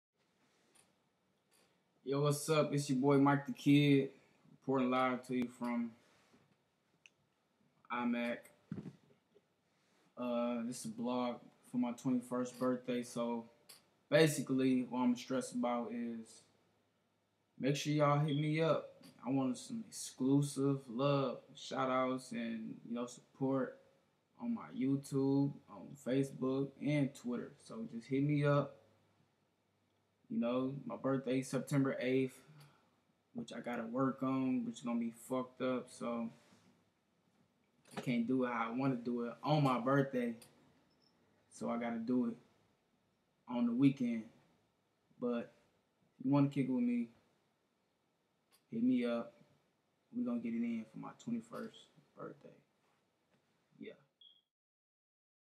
Mike Da Kidd-1day till 21st Birthday Blog
exlusive Blog from Mike Da Kidd 1day b4 his 21st Birthday.